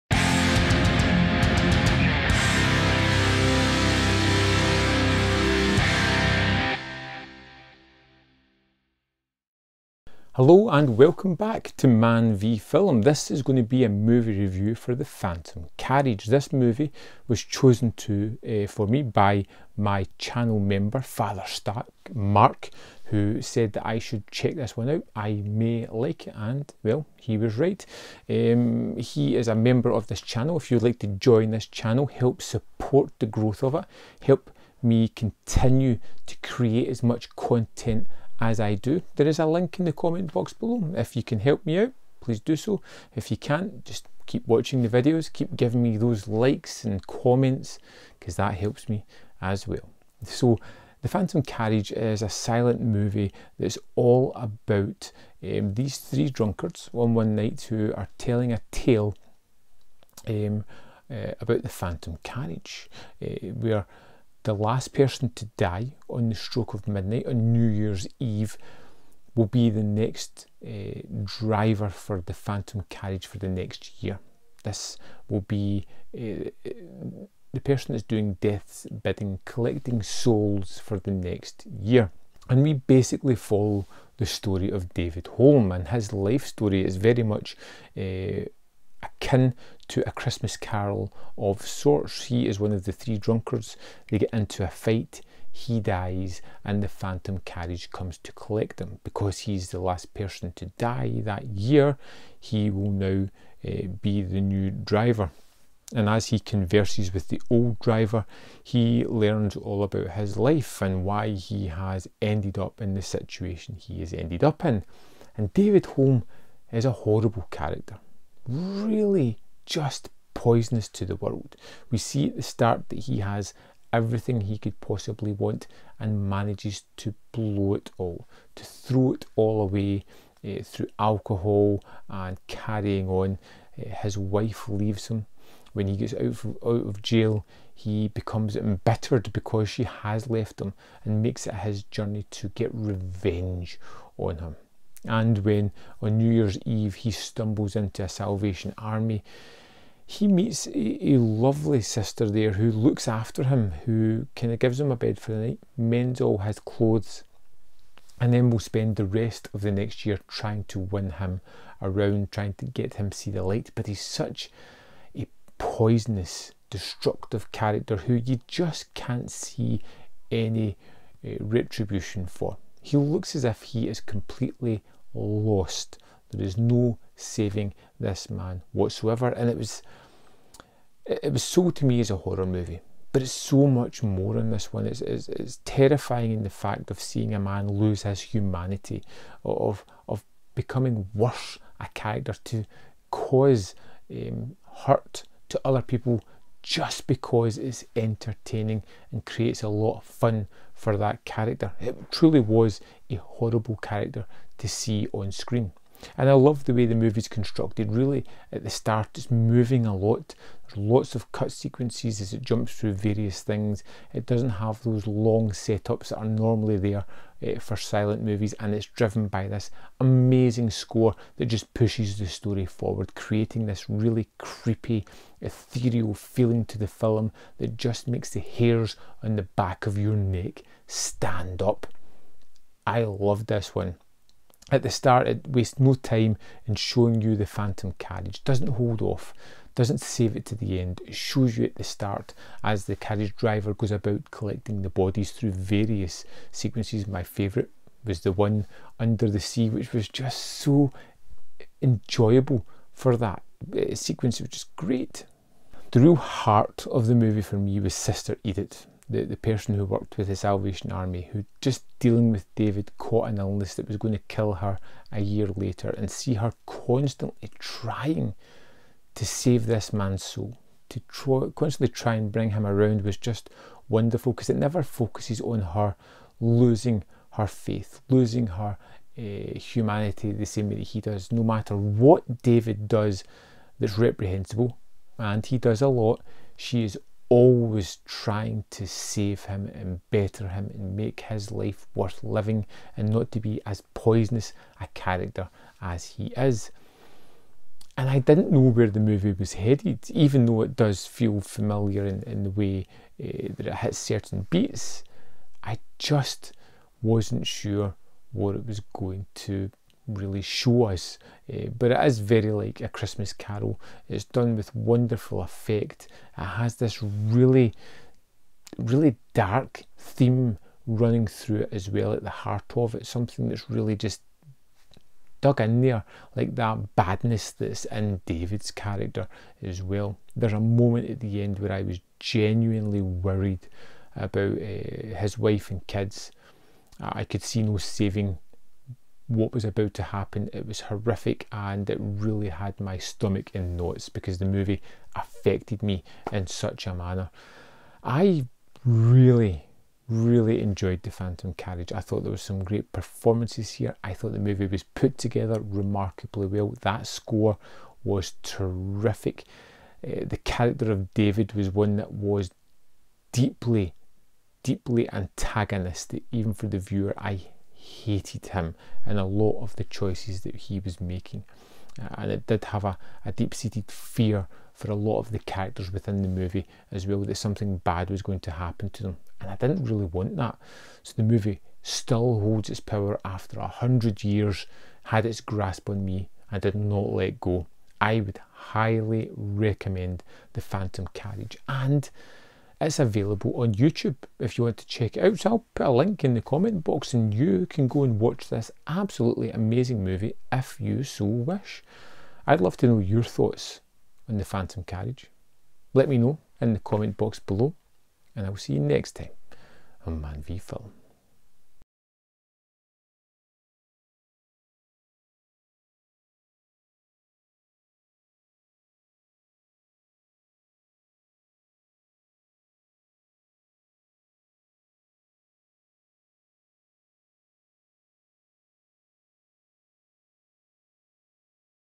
The Phantom Carriage | 1921 | Movie Review | Criterion Collection # 579 | Silent cinema
On New Year's Eve, the driver of a ghostly carriage forces a drunken man to reflect on his selfish, wasted life.

or

Movie Review for The Phantom Carriage